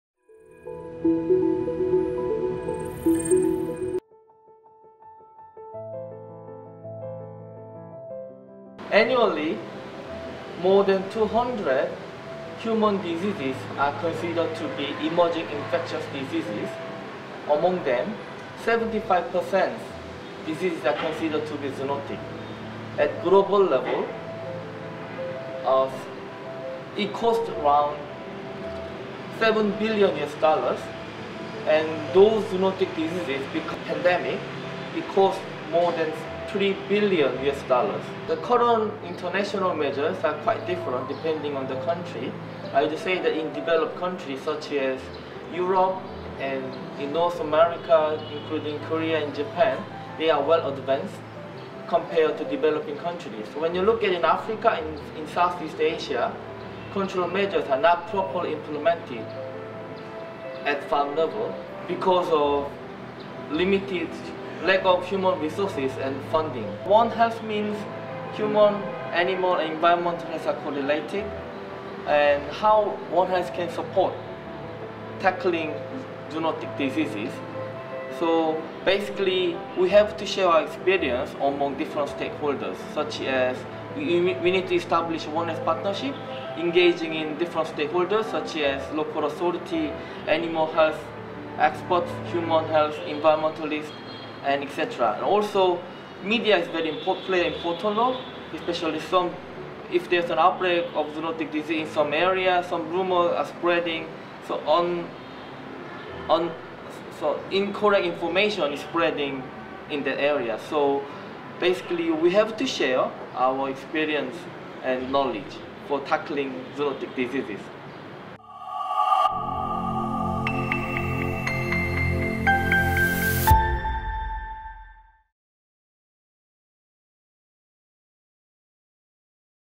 Controlling zoonotic diseases through a One Health approach
Hu Suk Lee, a veterinary epidemiologist at ILRI, explains how One Health approaches are use to control zoonotic diseases.